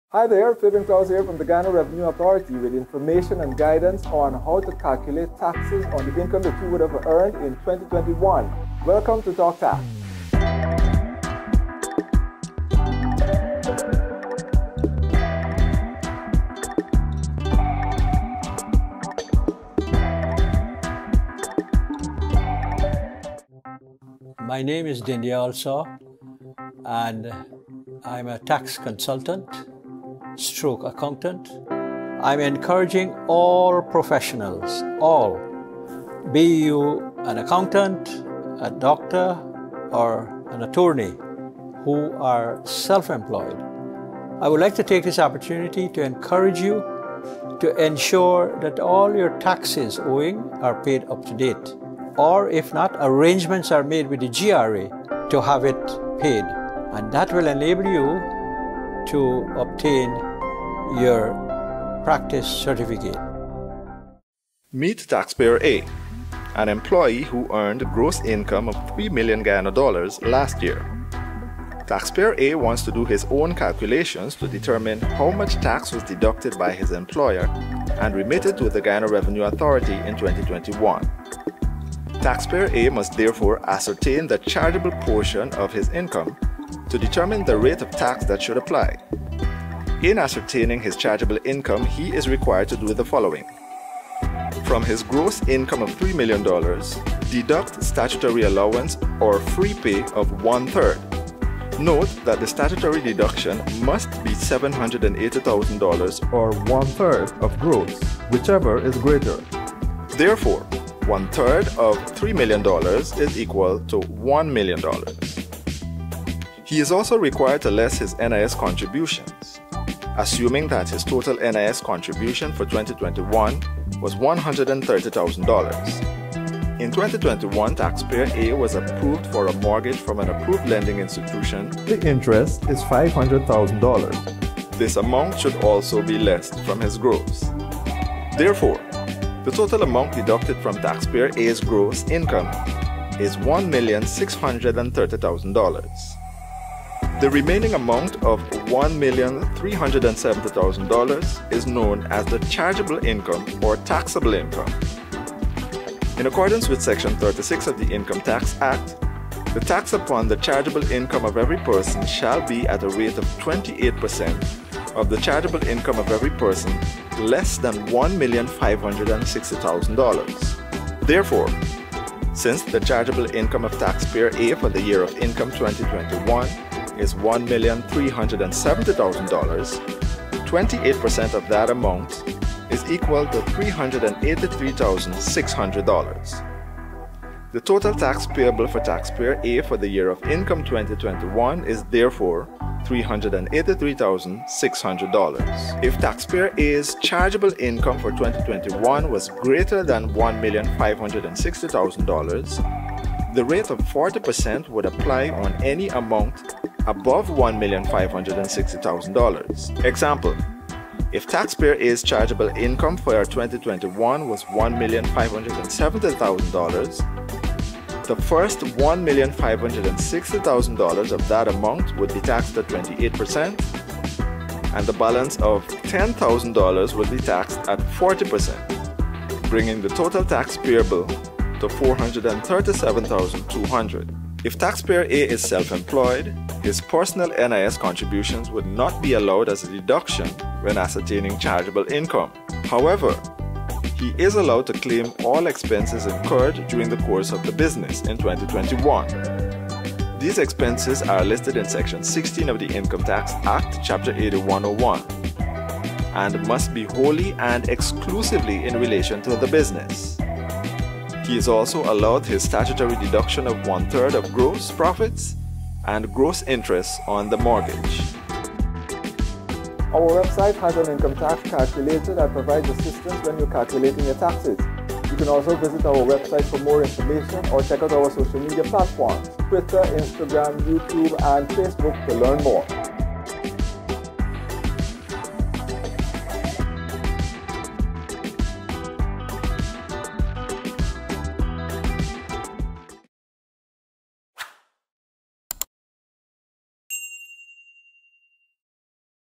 TalkTax Demo on Ascertaining The Chargeable Income
The April 30 deadline for the submission of Income Tax is quickly approaching, and we want to ensure you can properly determine your Income Taxes due.
Here's a quick demo on how you can calculate your Chargeable Income and total tax deductions.